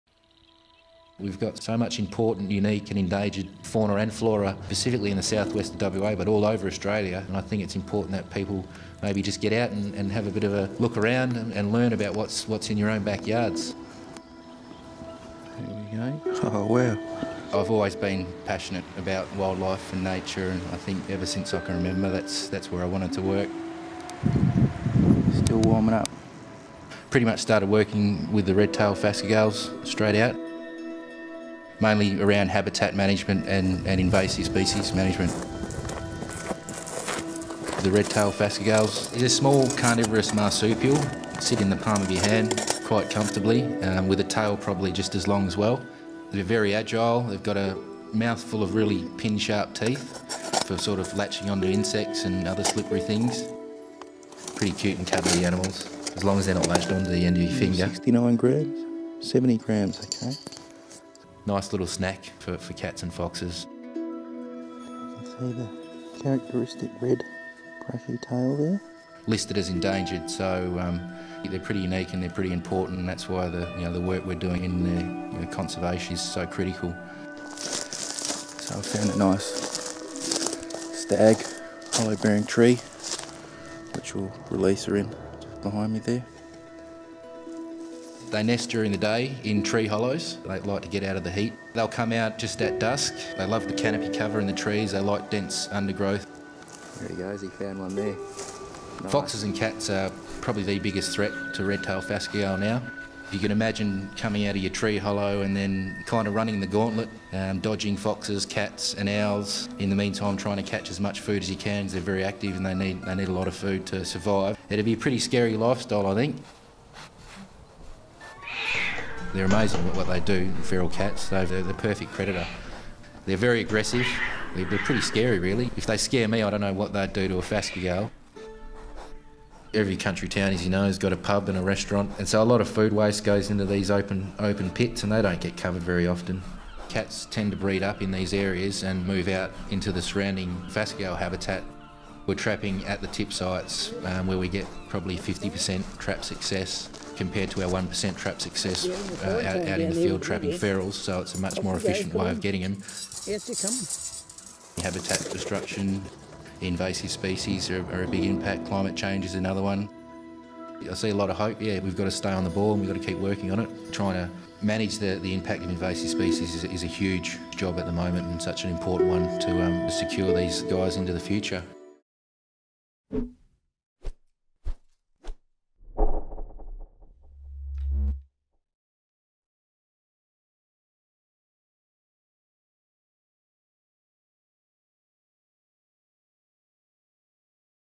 The plight of the phascogale
The life of the cute marsupial known as the Phascogale is "a short, hard one," according to biologist and photographer Leon Rakai.

"I'm a conservationist first and photographer second so my prime focus is the conservation of our endangered marsupials; in particular, the phascogale," Mr Rakai said.

The female phascogale lives for two or three breeding cycles, producing three of four young during each of the cycles, while the males typically only live for one breeding cycle.

"All they do is look for females; they will forget about eating, they forget about predators; it's all about breeding for them," said Mr
 Rakai.

The agile carnivore typically emerges from its tree hollow at dusk to feed and will jump from tree to tree, using the scattered undergrowth as cover from predators as it gathers enough insects and grubs to meet its high-energy needs.

Invasive species and land clearing have both taken a toll on phascogale numbers and Mr Rakai believes its anonymity adds more pressure to its already small numbers.

He said much of the world's attention was on what he called "Hollywood species", such as lions, tigers and pandas, but hoped his photography would help.

"Once people see some of these images, people will see the phascogale is a pretty cute and cuddly creature," Mr Rakai said.

"As long as it's not latched on to the end of your finger; they have a strong little bite to them."